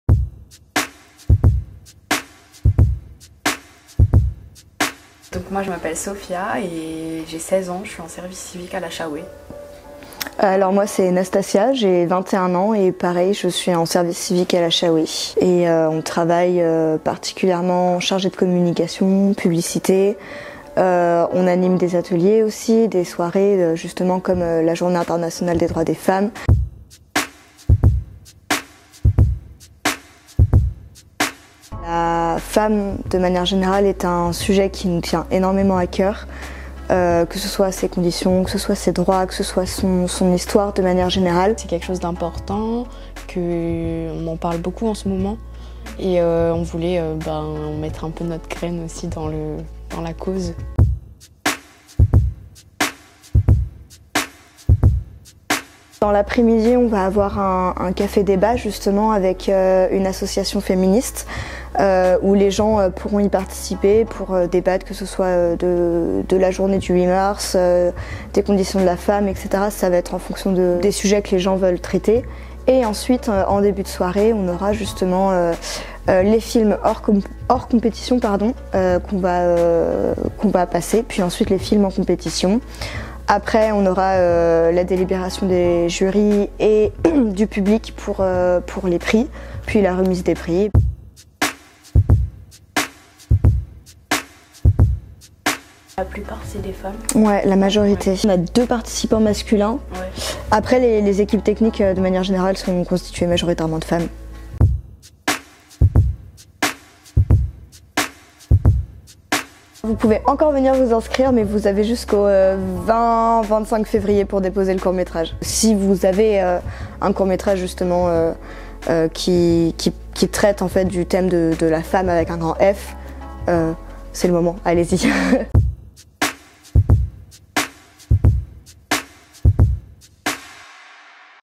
Interview : court-métrages pour la Journée Internationale des Droits des Femmes
Mardi 15 février, nous avions rendez-vous à la Chaouée pour rencontrer Nastassia Smaljovic et Sofia Khelifa, organisatrice d'un concours de court-métrages pour la Journée Internationale des Droits des Femmes.
Le festival aura lieu le 8 mars à partir de 17h30 à la Chaouée au 1 rue du Champé à Metz